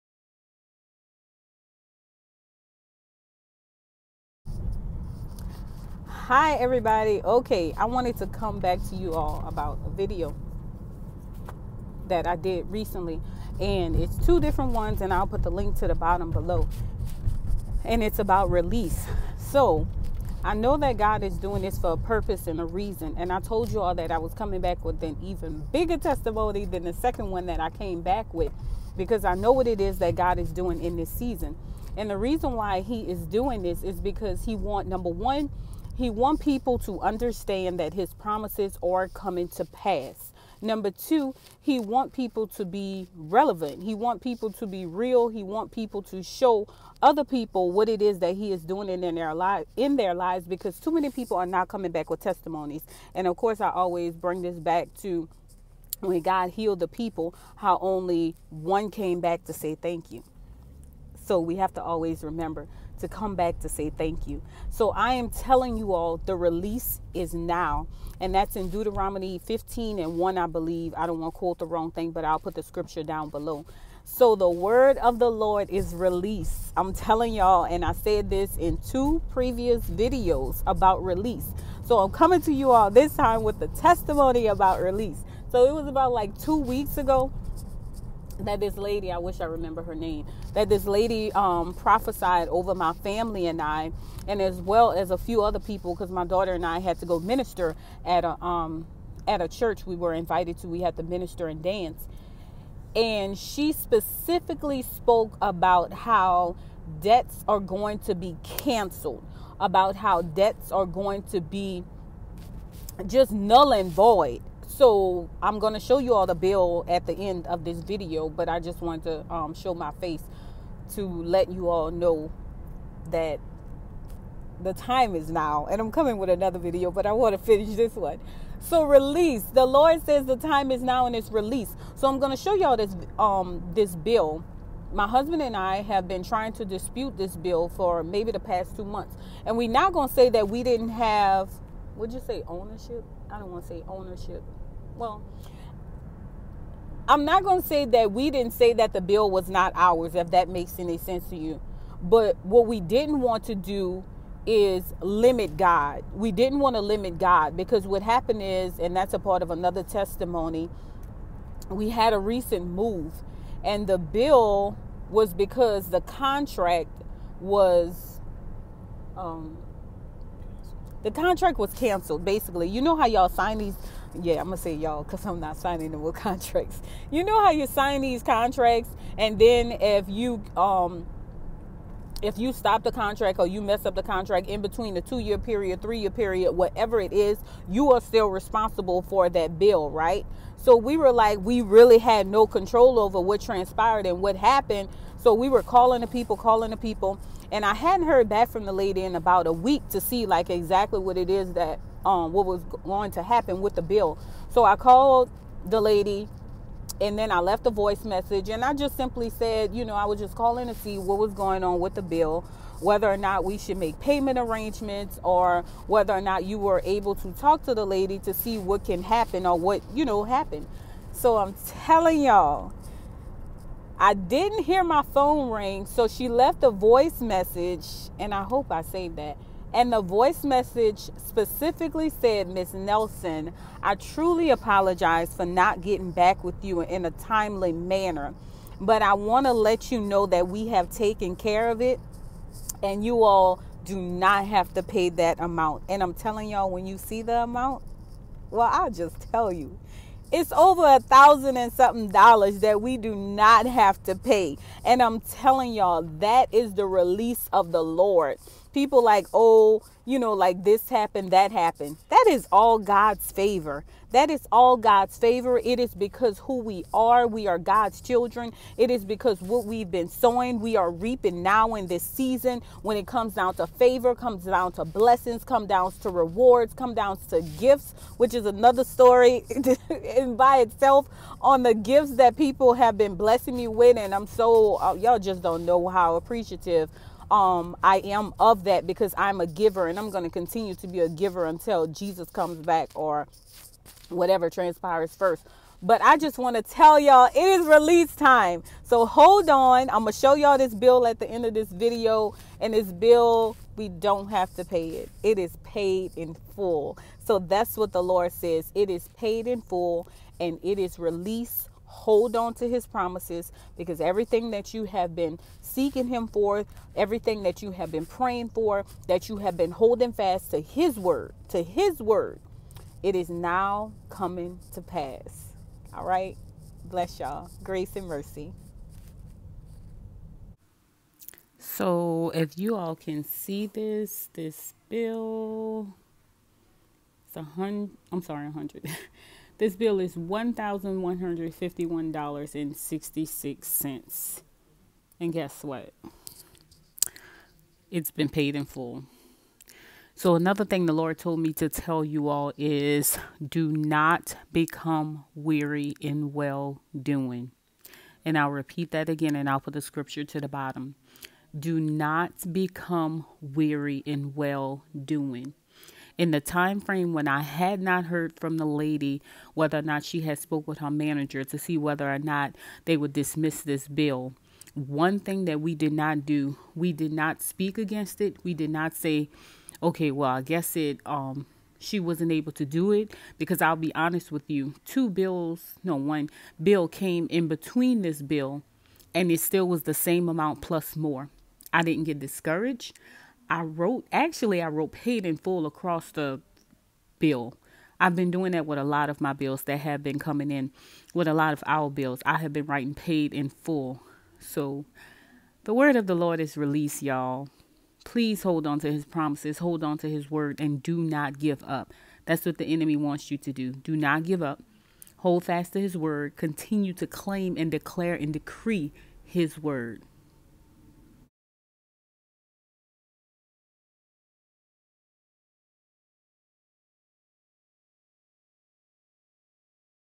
Release 3
“So let’s not get tired of doing what is good. At just the right time we will reap a harvest of blessing if we don’t give up.”
‭‭Galatians‬ ‭6:9‬ ‭NLT‬‬

Deuteronomy 15:1 (entire chapter)
Testimony!!!
All Prophesy should be confirmation from the Lord!
Make sure it lines up with His word!

*The contract was “broken” before our contract was up! (Correction)